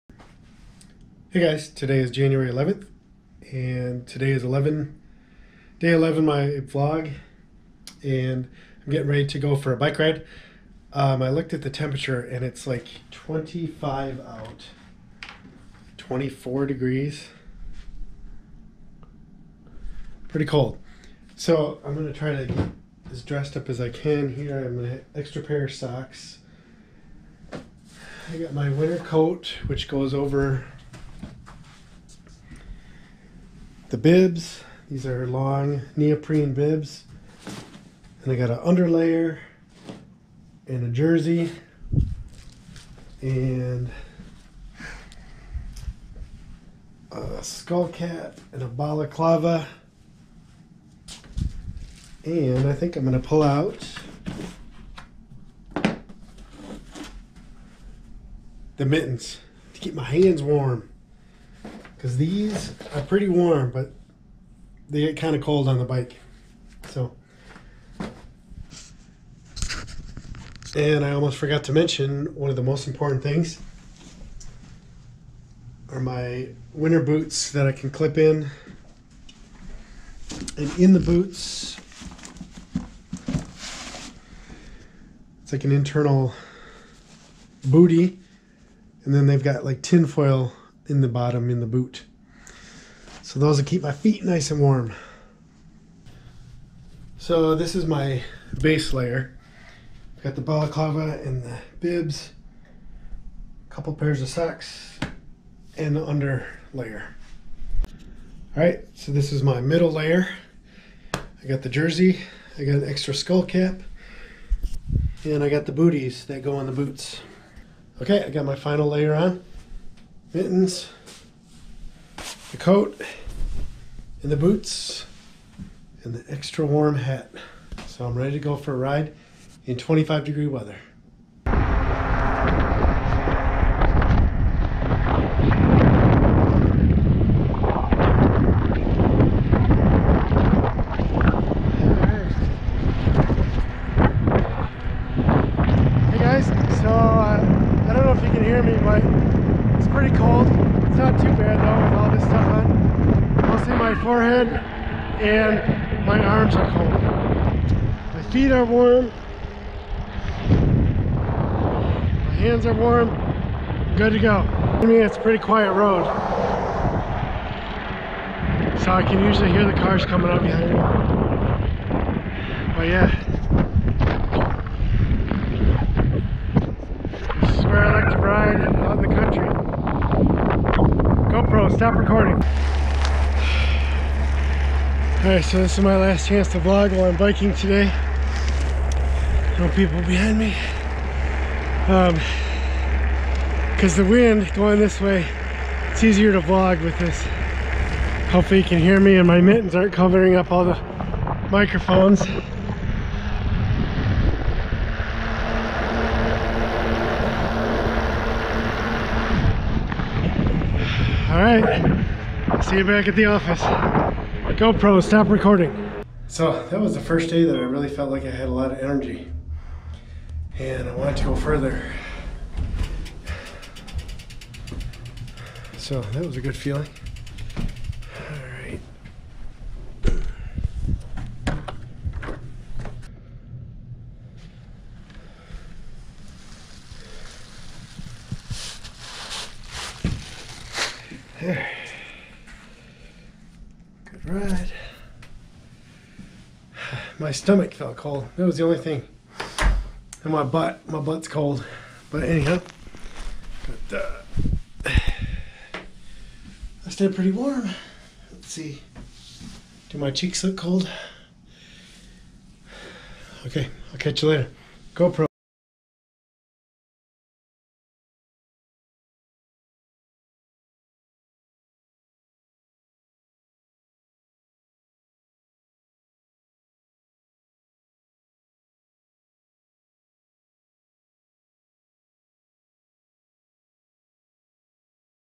What do I wear when I ride my bike in freezing temps?
I'll show you what I wear to stay warm when I go biking in freezing temps.  Check out the first couple of minutes of the video.